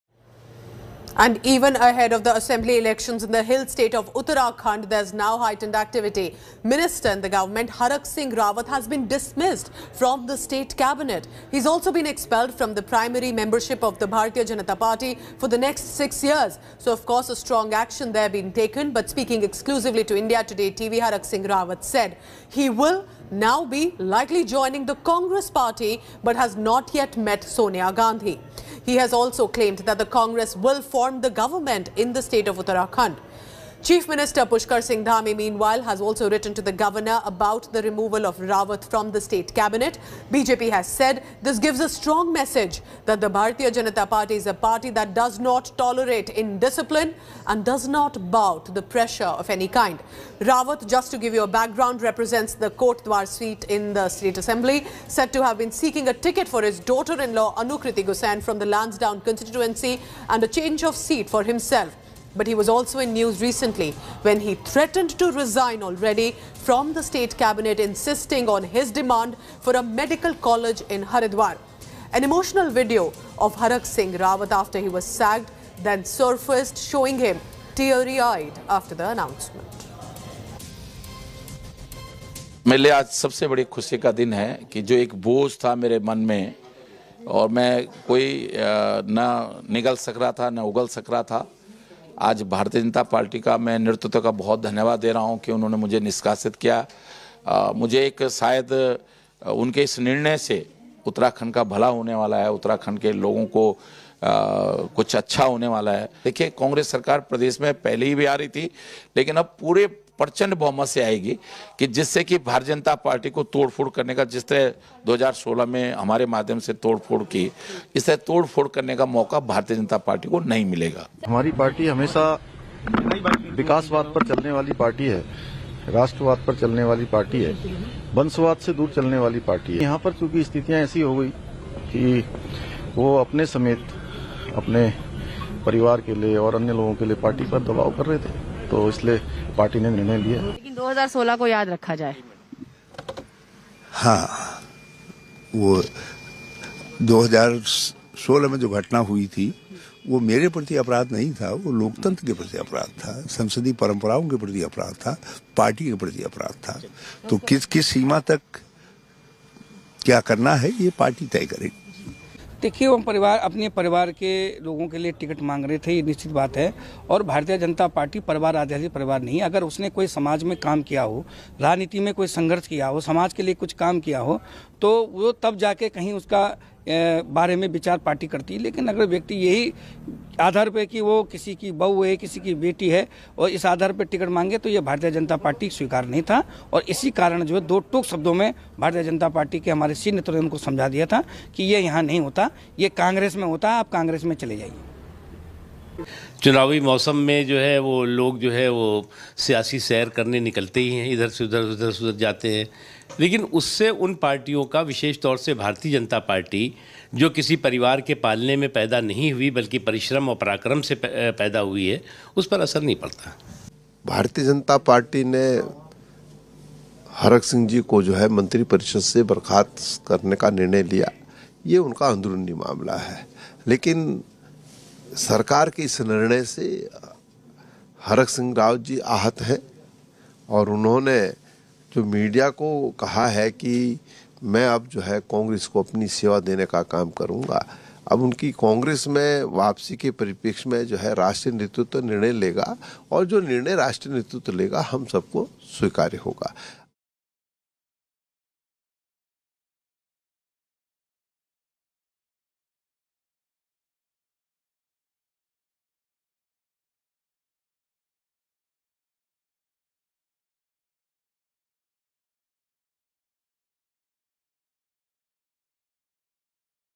BJP Expels Harak Singh Rawat For Anti-Party Activities In Uttarakhand, Minister To Join Congress
BJP on late Sunday night expelled Uttarakhand minister Harak Singh Rawat from the party for a period of six years. Chief Minister Pushkar Singh Dhami dismissed Rawat from his Cabinet for anti-party activities, finally ending a nearly month-long drama.

Chief Minister Pushkar Singh Dhami met him over lunch to agree to his demand for a medical college in his constituency, Kotdwar, after he threatened to quit. But Harak Singh Rawat's tantrums in seeking three tickets for his family, including his daughter-in-law Anukriti, and alleged secret meetings with the Congress leaders in Delhi was the final nail in the coffin.